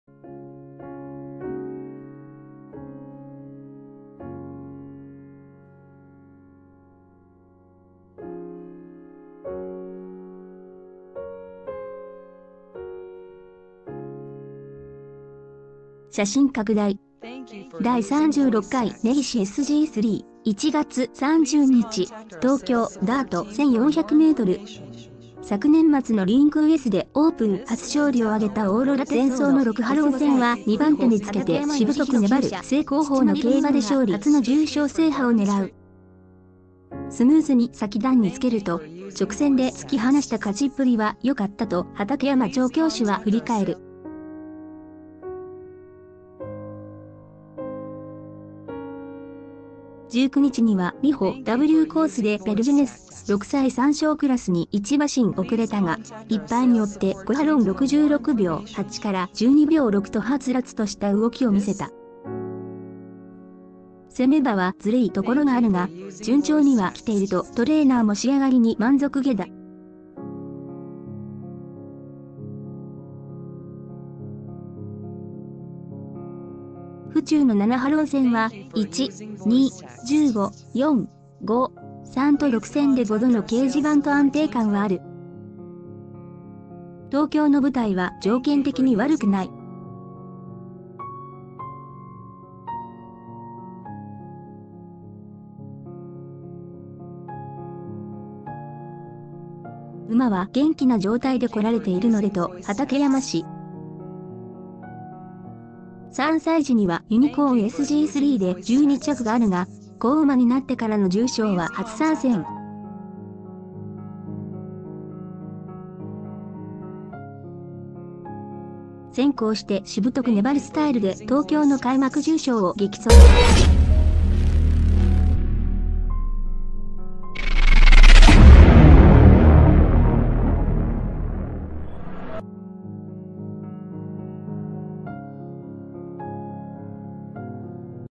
【根岸Ｓ】６戦ぶり府中で重賞初制覇狙うオーロラテソーロ 畠山調教師「馬は元気」|Top News
【根岸Ｓ】６戦ぶり府中で重賞初制覇狙うオーロラテソーロ 畠山調教師「馬は元気」|Top News 写真拡大 ◆第３６回根岸Ｓ・Ｇ３（１月３０日、東京・ダート１４００メートル） 昨年末のりんくうＳでオープン初勝利を挙げたオーロラテソーロ（牡５歳、美浦・畠山吉宏厩舎、父マリブムーン）が初の重賞制覇を狙う。 前走の６ハロン戦は２番手につけてしぶとく粘る正攻法の競馬で勝利。「スムーズに先団につ